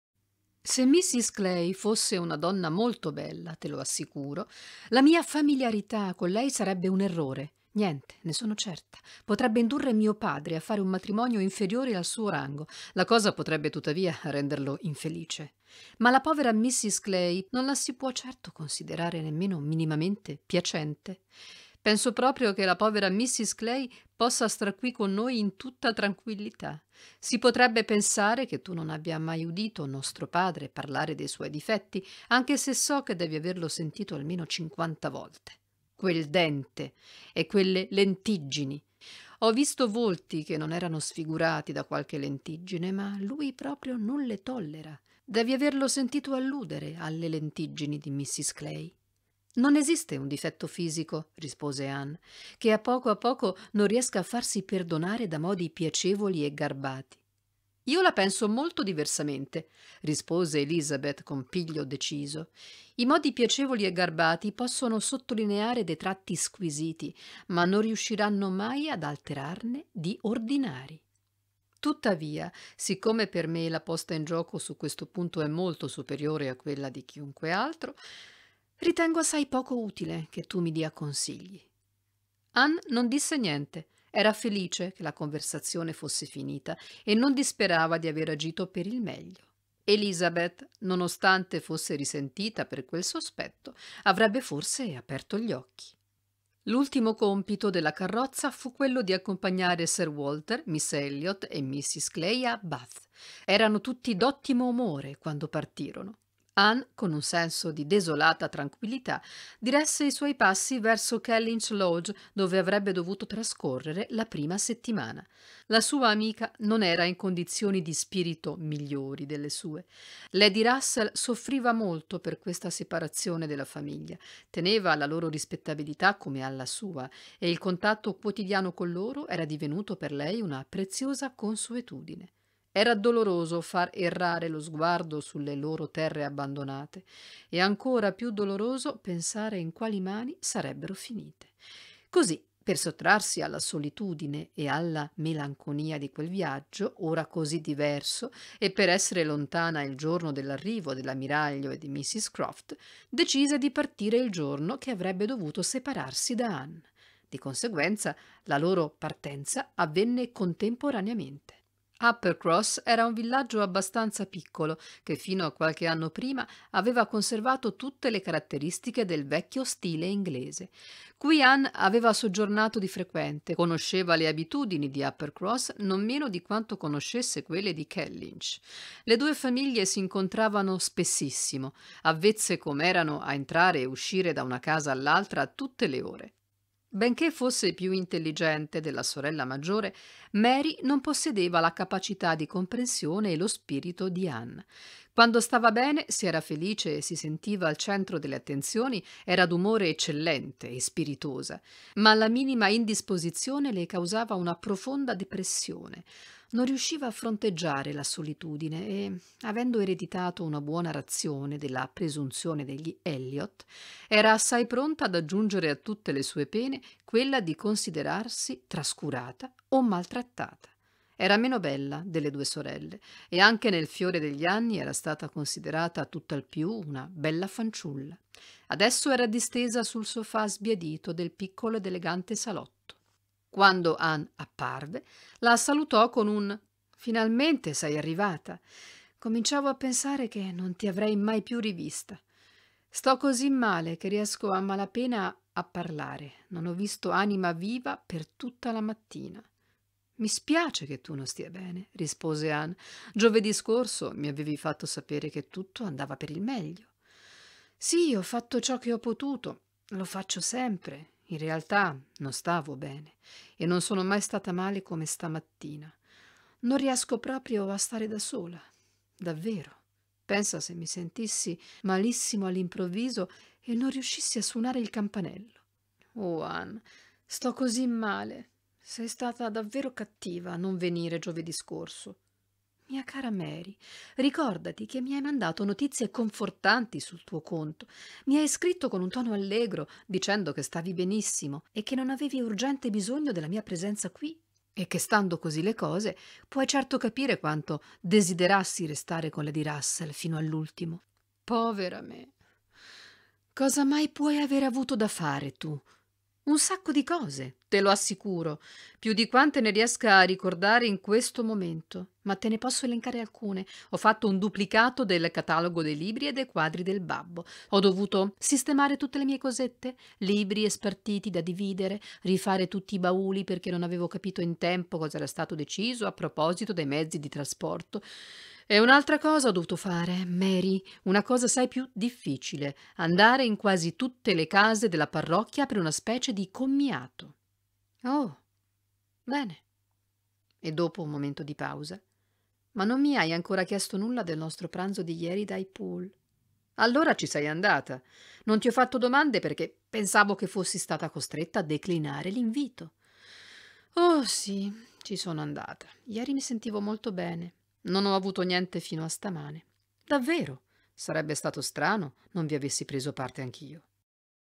Jane Austen Persuasione 2
Michela Mazzarini voce narrante.
Tratto dal romanzo "Persuasione" di Jane Austen.